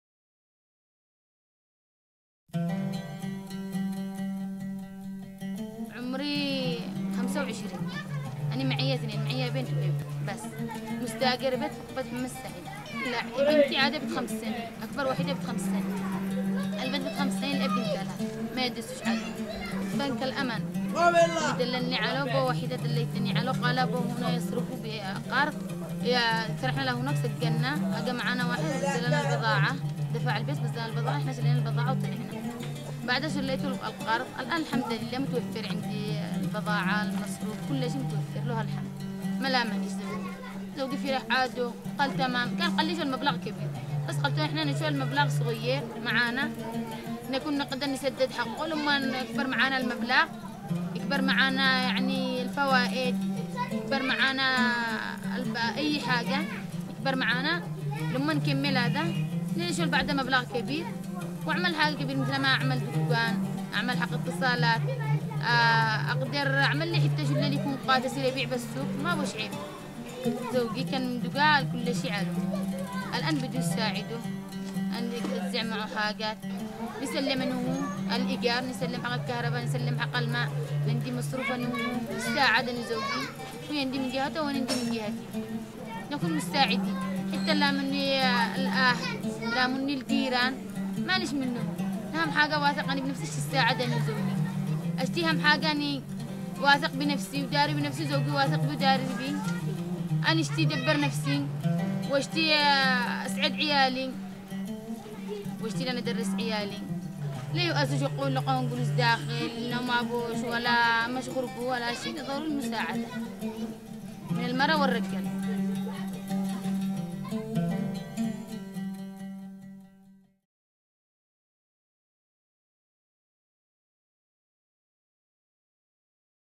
Microfinance in Yemen: Amira's Story
In Yemen 75% of the population is under the age of 25 and the average yearly income is USD 1,100. In 2009, Silatech and Al-Amal Microfinance bank partnered to launch the first youth microfinance fund in the Middle East, supporting youth to start or grow their businesses. In this video, Amira explains how the loan she received through the program has allowed her to contribute through her small business to her family's prosperity.